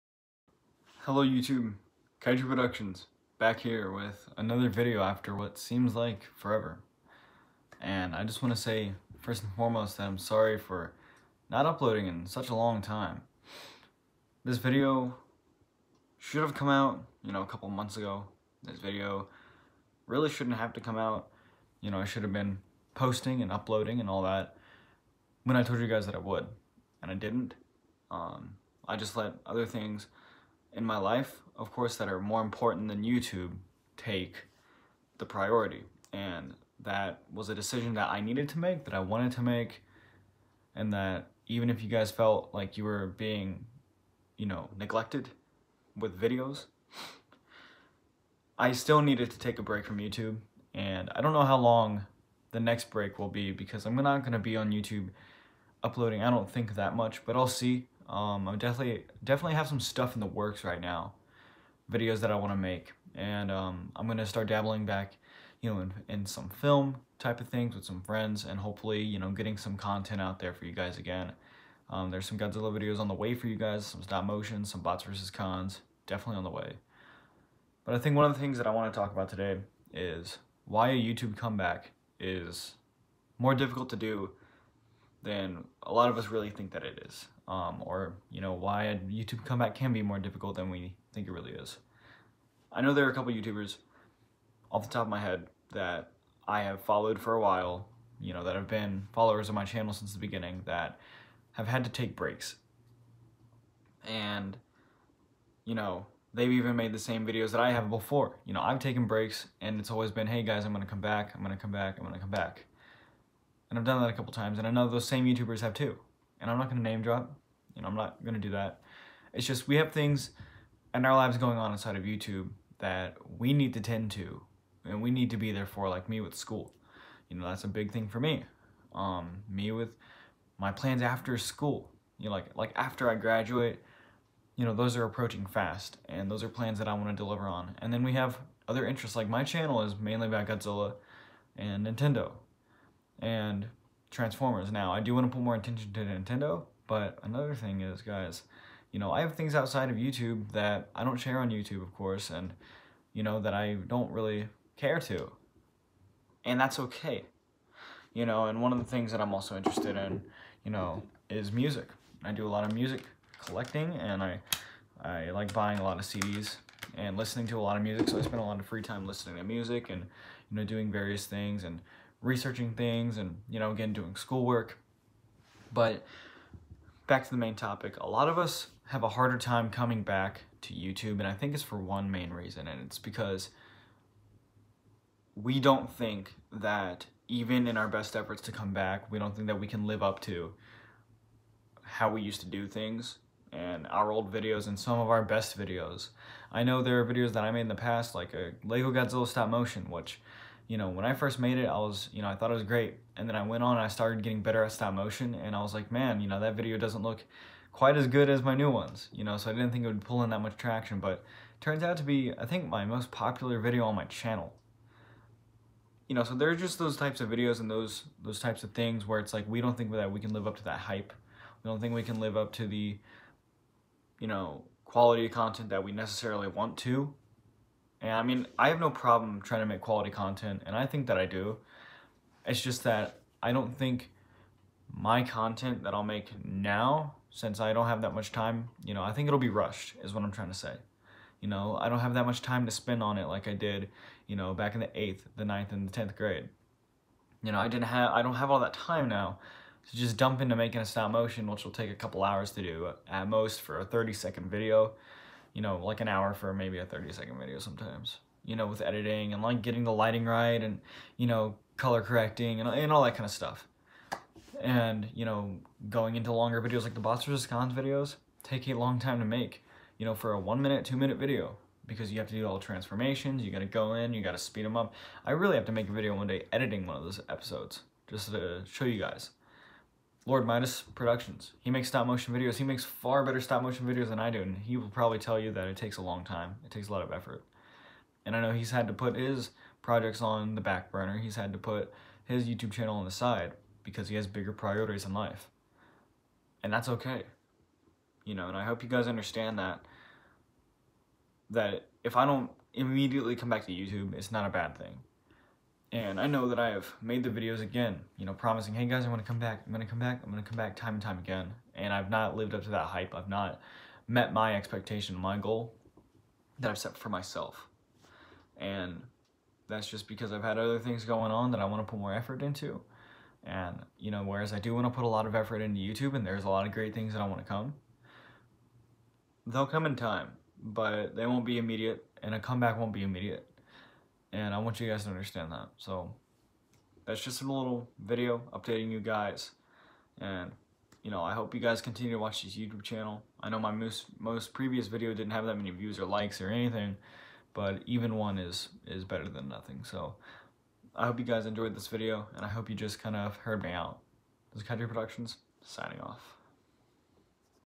Why YouTube Comebacks can be HARD
This is just an update video for those who still watch. I am making this video now after seeing a Luis Duran video on a comeback in the future